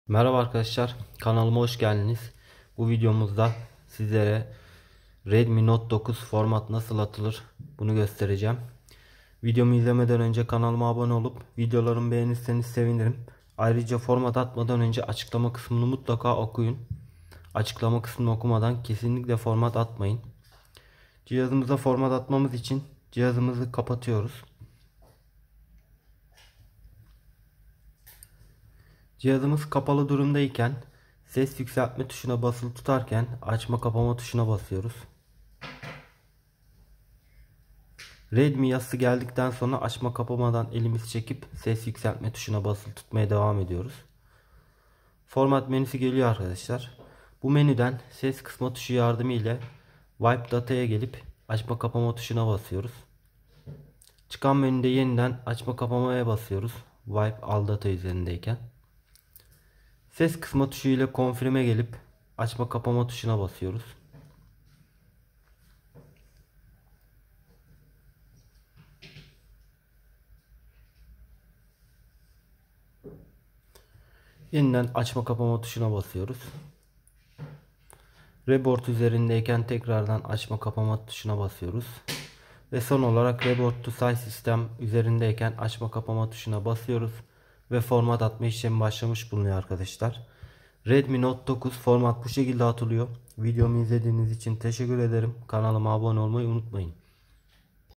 Xiaomi Redmi Note 9 Format Atma - Hard Reset - Sıfırlama - Desen Kilidi Açma 🇹🇷
Xiaomi Redmi Note 9 Format Atma - Hard Reset - Sıfırlama

Format İşlemini Yapmadan Önce , Ayarlar Kısmına Girerek, Hesaplar Kısmından Googleyi Seçiyoruz Ve Google Hesabını Kaldırıyoruz. Kaldırmadan Yaparsak , Bu Cihaz Sıfırlandı Önceden Senkonize Edilen Google Hesabınızı Ve Şifrenizi Girin Diye Bir Hata Verir. Hesap Ve Şifreyi Bilmiyorsanız Yazılım Atılmak Zorunda Kalınır.Cihazımızda Ekran Kilidi Varsa Kapatamıyorsak , Şarjının Bitmesini Bekleyiniz Ve Cihaz Kapalı Durumda İKEN Şarjını Doldurunuz. Şarjı Doldurduktan Sonra Videodaki İşlemleri Yapabiliriz.